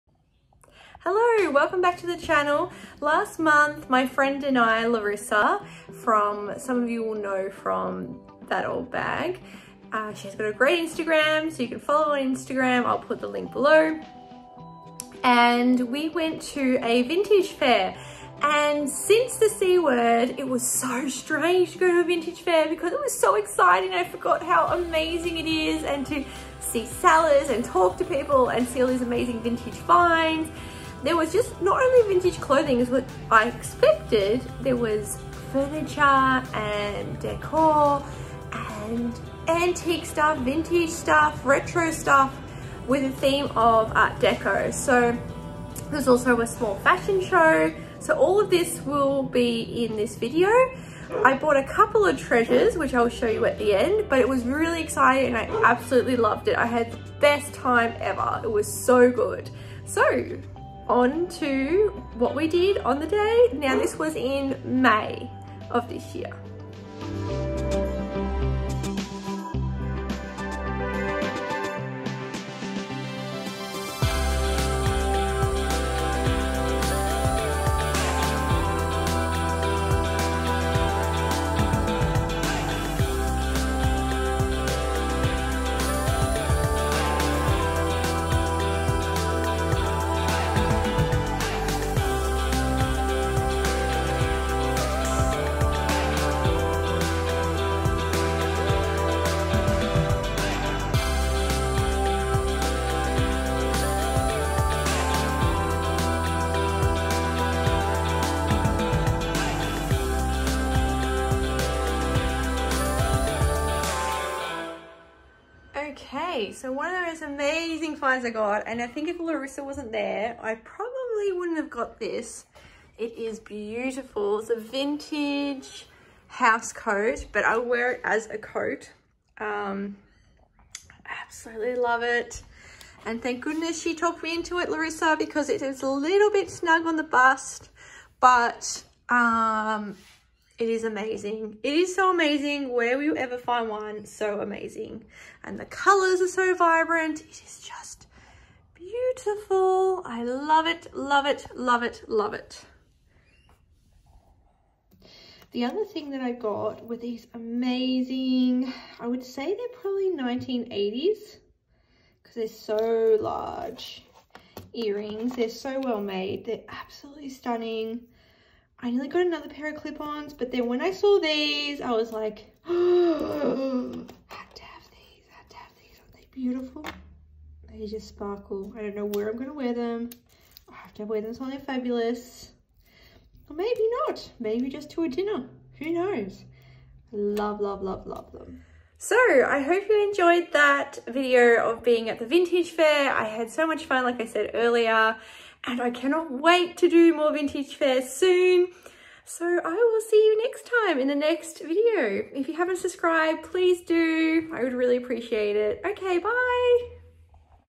Fun day out at Sydney vintage fair
Last month ( May 2021) my good friend Larissa and I went to the vintage fair in Sydney. We had a blast!

He’s a short clip of our day and some treasures we bought. :)